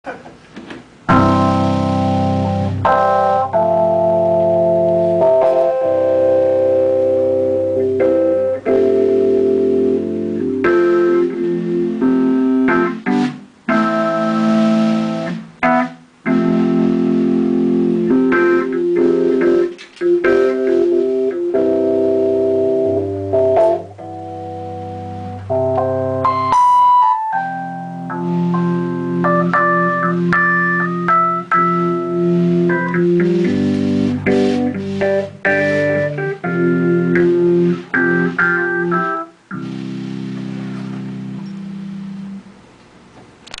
Vintage 1974 Rhodes Mark I Stage Electric Piano 88 Key Demo
This is a Mark I Stage Rhodes piano.  It is missing the legs and the sustain pedal.